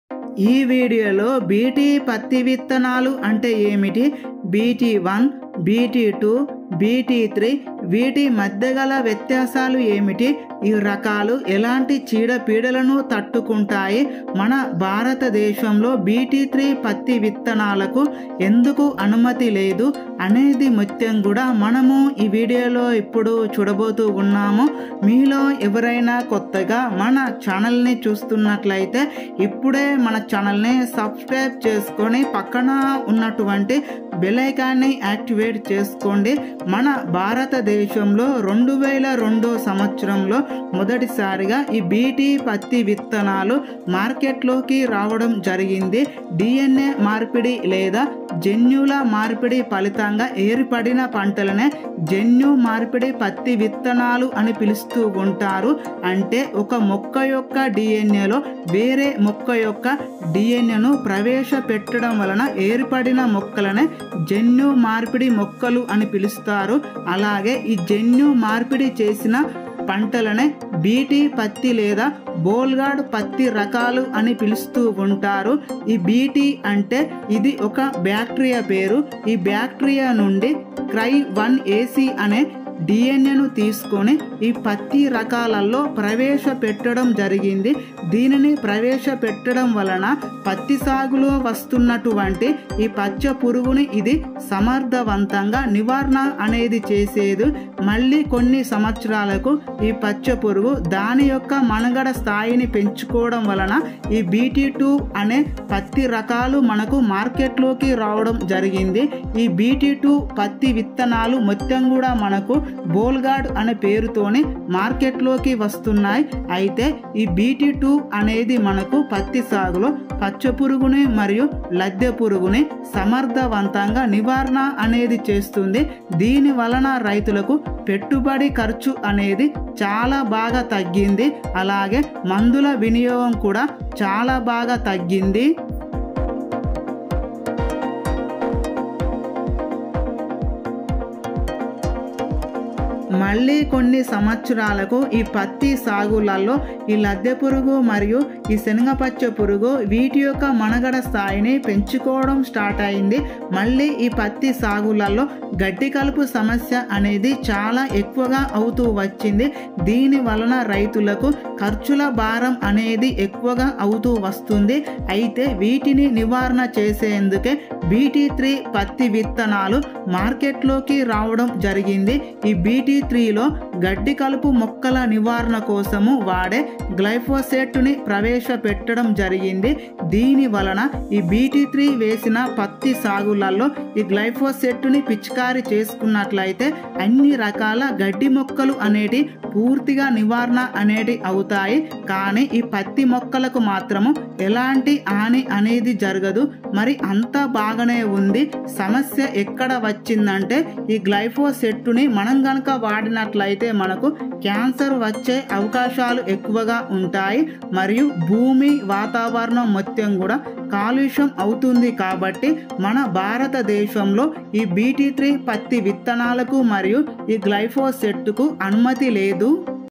BT - 1, BT - 2, BT - 3 పత్తి విత్తనాలు అంటే ఏమిటి.? | BT - 3 cotton seeds Full Details in telugu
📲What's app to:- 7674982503